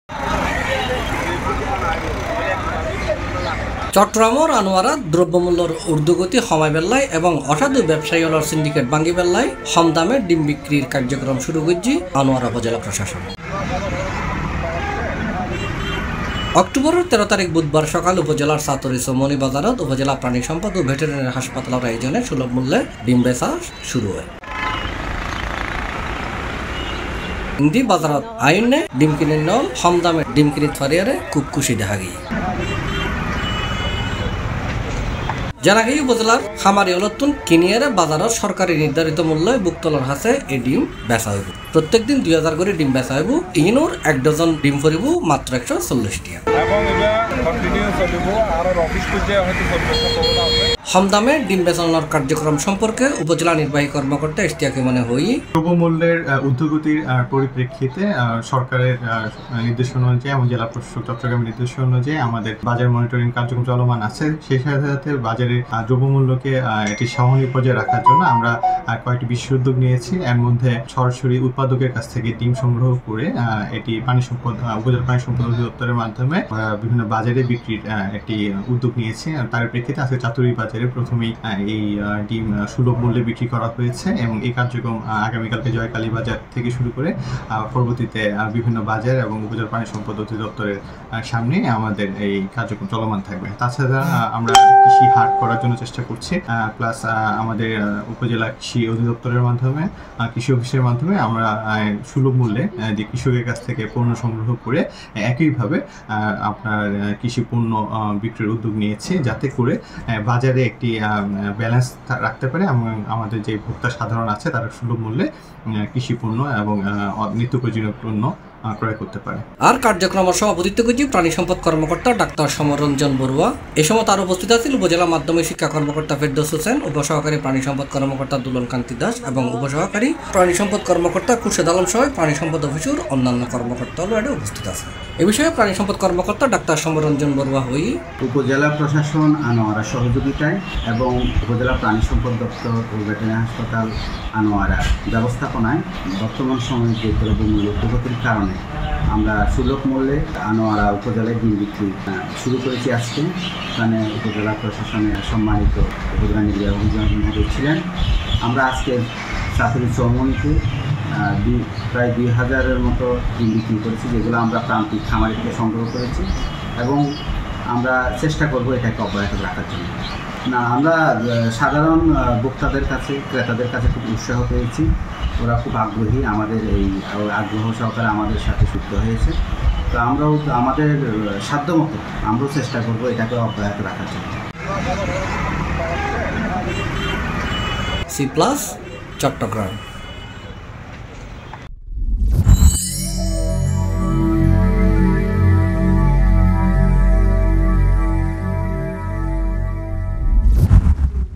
আনোয়ারায় দ্রব্যমূল্যের ঊর্ধ্বগতি রোধে সুলভ মূল্যে ডিম বিক্রি | Cplus
আনোয়ারায় দ্রব্যমূল্যের ঊর্ধ্বগতি রোধে সুলভ মূল্যে ডিম বিক্রি

Slogan: আরাঁ চাটগাঁইয়া হতা হই
Country: Bangladesh
Language: Bengali।Chittagong Language
Broadcast area: Online